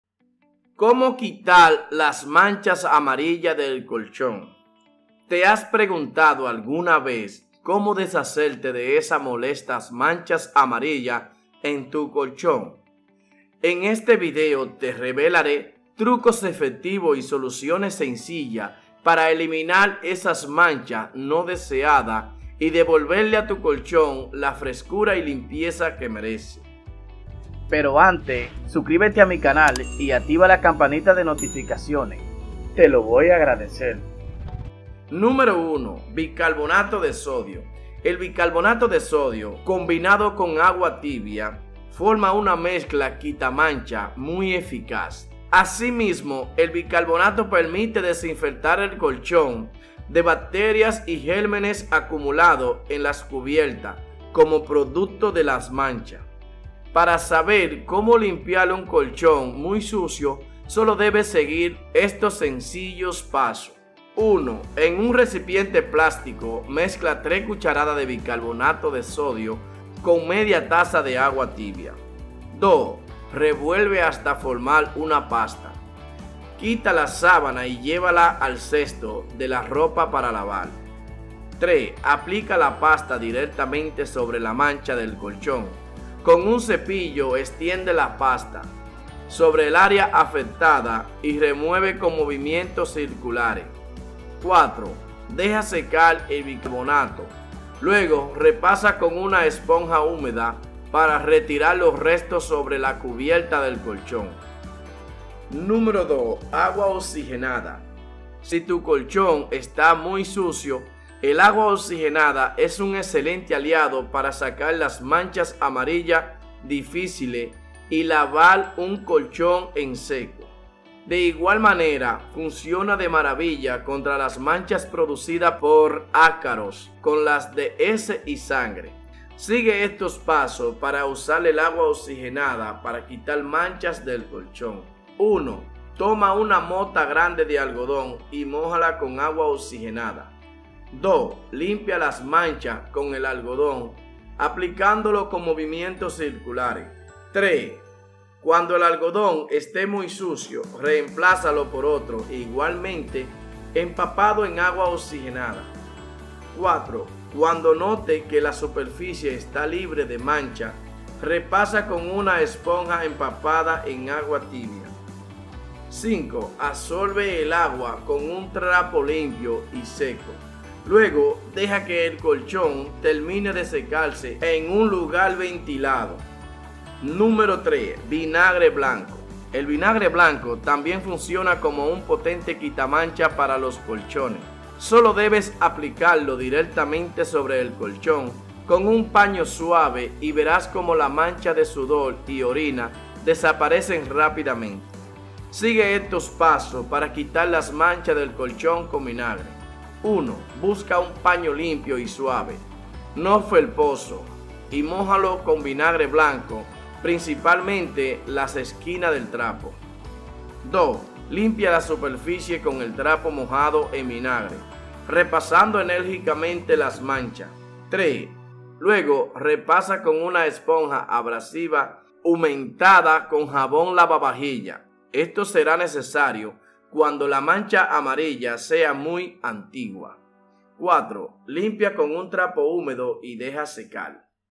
Cómo quitar las manchas amarillas del colchón
"¿Te has preguntado alguna vez cómo deshacerte de esas molestas manchas amarillas en tu colchón?

En este video, te revelaré trucos efectivos y soluciones sencillas para eliminar esas manchas no deseadas y devolverle a tu colchón la frescura y limpieza que merece.

1 Bicarbonato de sodio
El bicarbonato de sodio, combinado con agua tibia, forma una mezcla quita manchas muy eficaz.

 Asimismo, el bicarbonato permite desinfectar el colchón de bacterias y gérmenes acumulados en la cubierta como producto de las manchas.

Para saber cómo limpiar un colchón muy sucio solo debes seguir estos sencillos pasos.

1.En un recipiente plástico mezcla 3 cucharadas de bicarbonato de sodio con media taza de agua tibia.

2.Revuelve hasta formar una pasta.
Quita las sábanas y llévalas al cesto de la ropa para lavar.

3.Aplica la pasta directamente sobre las manchas del colchón. Con un cepillo extiende la pasta sobre el área afectada y remueve con movimientos circulares.

4.Deja secar el bicarbonato. Luego, repasa con una esponja húmeda para retirar los restos sobre la cubierta del colchón.

2 Agua oxigenada
Si tu colchón está muy sucio, el agua oxigenada es un excelente aliado para sacar las manchas amarillas difíciles y lavar un colchón en seco.

De igual manera, funciona de maravilla contra las manchas producidas por ácaros, como las de heces y sangre.

Sigue estos pasos para usar el agua oxigenada para quitar manchas del colchón:

    1. Toma una mota grande de algodón y mójala con agua oxigenada.

    2. Limpia las manchas con el algodón, aplicándolo con movimientos circulares.

    3. Cuando el algodón esté muy sucio, reemplázalo por otro, igualmente empapado en agua oxigenada.

    4. Cuando notes que la superficie está libre de manchas, repasa con una esponja empapada en agua tibia.